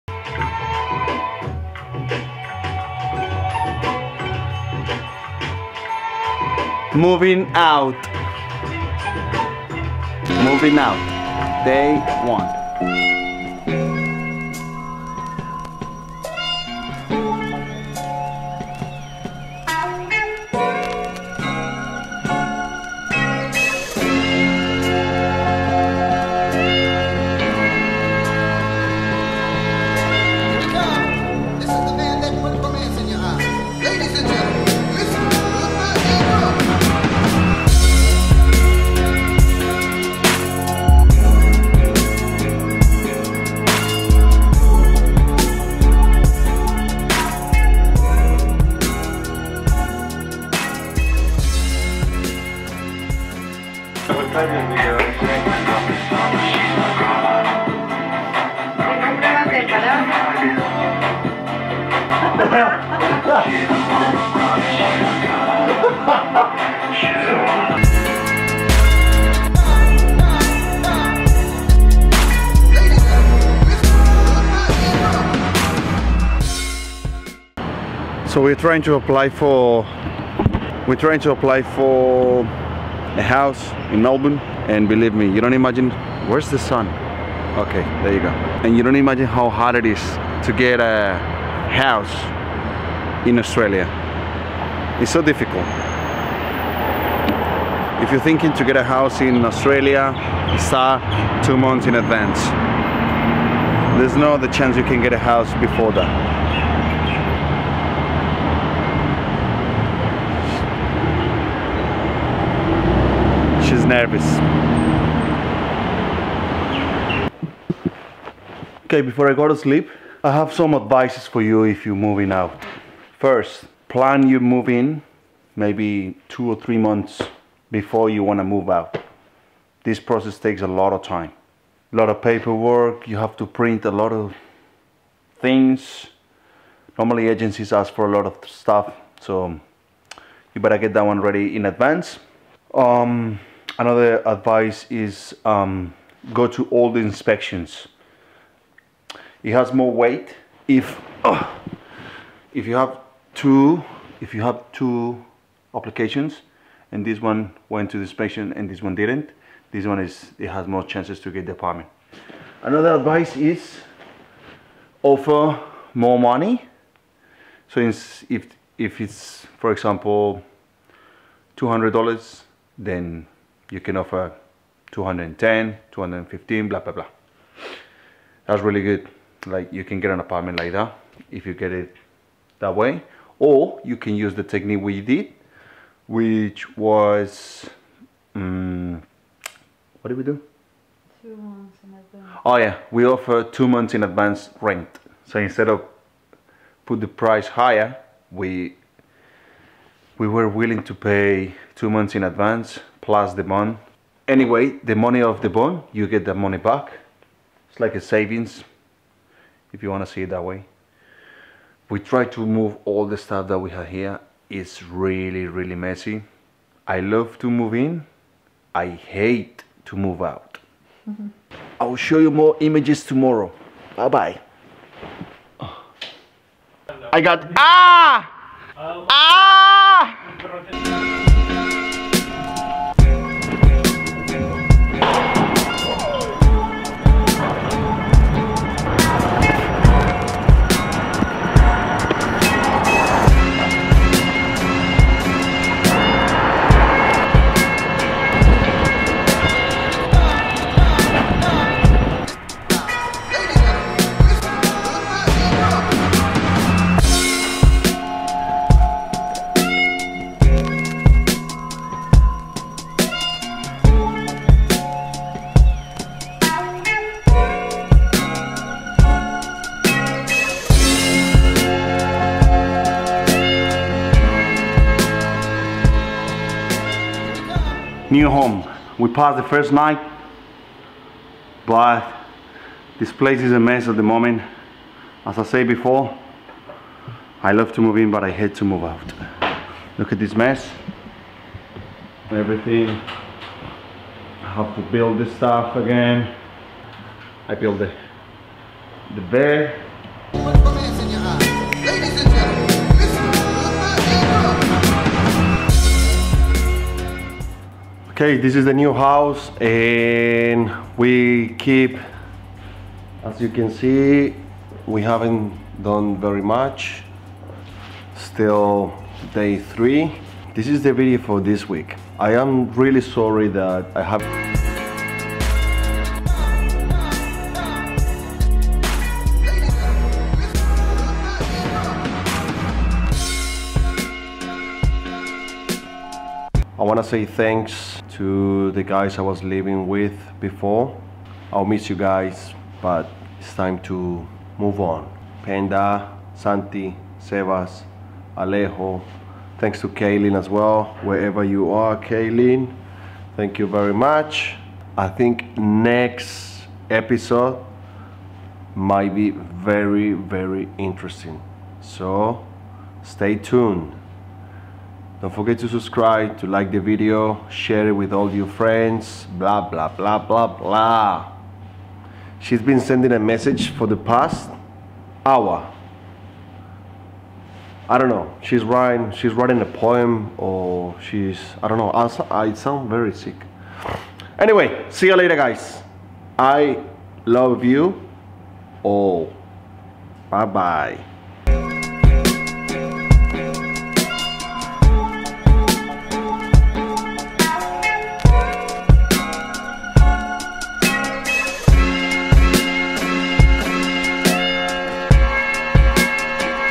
How To Get an Apartment in Australia
Hi Everyone. Let me show you How to Ger an Apartment in Australia. I know I've been away for a while and I'm sorry for that. In this video I share my own experience about how we got the lease approved. Please bear in mind that this advice might not work for every single person but it can give you more chances to get leases.

Thanks for all the support everyone had Given me all this time.

That helps me a lot and I don't think it'll take much effort to you.